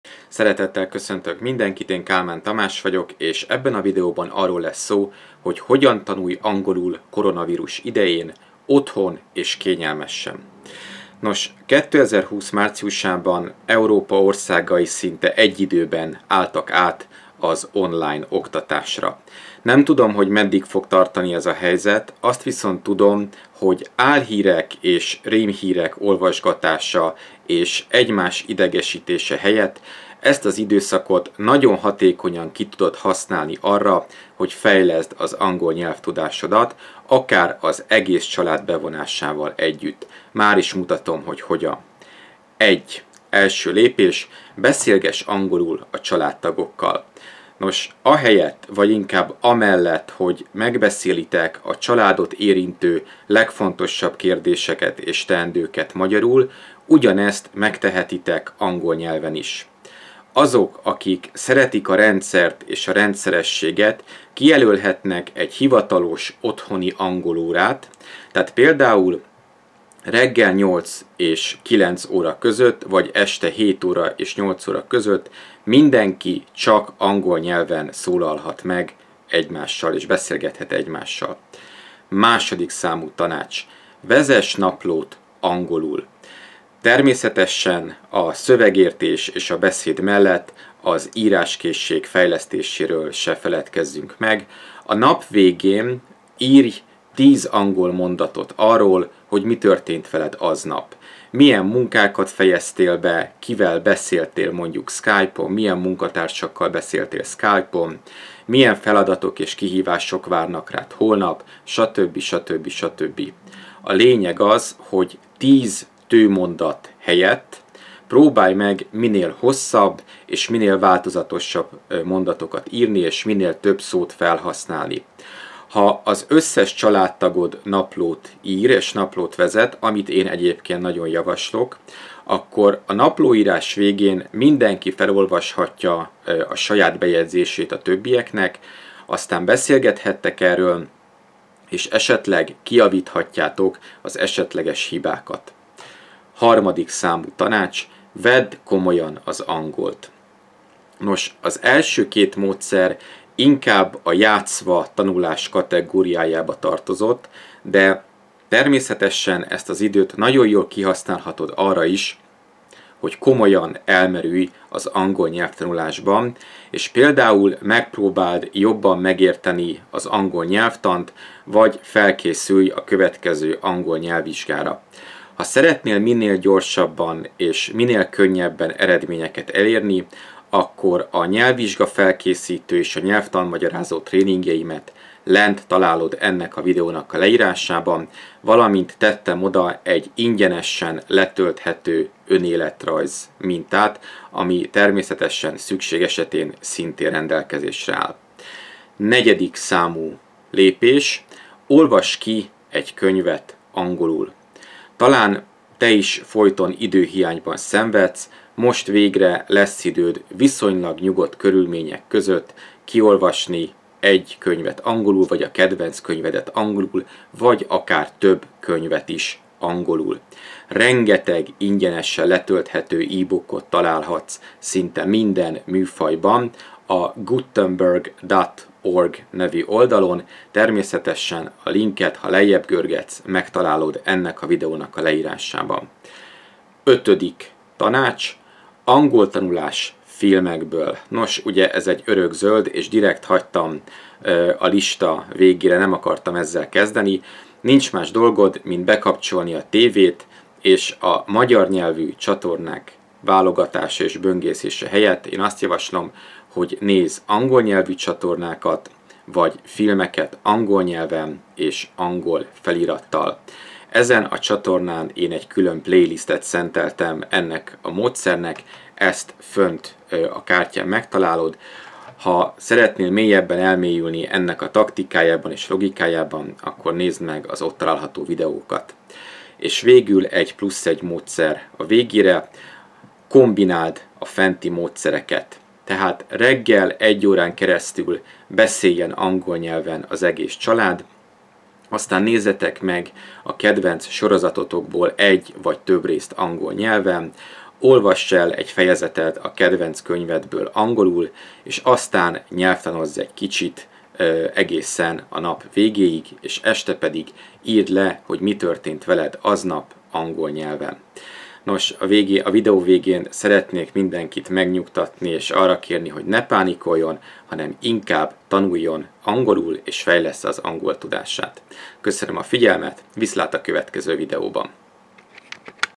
Angol Tanulás Koronavírus idején: Otthon, Kényelmesen! (5+1 módszer)
Az angol nyelvtan – Érthetően! - Otthoni Online Tréning

2020 márciusában Európa országai szinte egyidőben álltak át az online oktatásra. Nem tudom meddig tart ez a helyzet, viszont az tudom, hogy álhírek és rémhírek olvasgatása és egymás idegesítése helyett ezt az időszakot nagyon hatékonyan ki tudod használni arra, hogy fejleszd az angol nyelvtudásodat, akár az egész család bevonásával.

Mutatom hogyan:

1 Beszélgess angolul a családtagokkal

Ahelyett, vagy amellett, hogy megbeszélitek a családot érintő legfontosabb kérdéseket magyarul, ugyanezt megtehetitek angol nyelven is. Azok, akik szeretik a rendszert, kijelölhetnek egy hivatalos otthoni „angol órát”. Tehát például reggel 8-tó 9-ig, vagy este, 7-től 8-ig mindenki csak angol nyelven szólalhat meg.

2 Vezess naplót angolul

Természetesen a szövegértés és a beszéd mellett az íráskészség-fejlesztéséről sem feledkeztem meg. A nap végén írj 10 angol mondatot, hogy mi történt veled aznap. Milyen munkát fejesztél be, mi vár rád holnap, stb. A lényeg, hogy 10 tőmondat helyett próbálj meg minél hosszabb és változatosabb mondatokat írni. Ha az összes családtag ír naplót, akkor az írás végén mindenki olvassa fel a saját bejegyzését a többieknek, aztán beszélgethettek erről és kijavíthatjátok az esetleges hibákat is.

3 Vedd komolyan az angol tanulást

Az első 2 módszer inkább a játszva tanulás kategóriájába tartozott.
De természetesen kihasználhatod ezt az időt arra is, hogy komolyan elmerülsz az angol tanulásban, és például megpróbálod jobban megérteni az angol nyelvtant, vagy felkészülsz a nyelvvizsgára. Ha szeretnél minél gyorsabban és könnyebben eredményeket elérni, akkor a nyelvvizsga-felkészítő és nyelvtan-magyarázó tréninget, valamint az ingyenesen letölthető önéletrajz mintát a videó leírásában találod.

4 Olvass ki egy könyvet angolul
Talán te is folyton időhiányban szenvedsz, de most végre lesz időd viszonylag nyugodt körülményke között kiolvasni egy könyvet angolul, Rengeteg ingyenesen letölthető ebook-t találsz szinte minden műfajban:

5 Angol tanulás filmekből

Kapcsold be a tv-t, de csak angol nyelvű csatornákat és filmeket nézz.
Ezen a csatornán egy egész sorozatot szentelek ennek a módszernek. Részletekért kattints fent a playlistre.

+1 Módszer

Kombináld a fenti módszereket, tehát reggel 1 órán keresztül beszéljen az egész család angolul, aztán nézz meg egy vagy több részt a kedvenc sorozatodból angol nyelven, olvass el egy fejezetet egy jó könyvből, aztán nyelvtanozz egy kicsit és este írd le angolul, hogy mi történt veled aznap.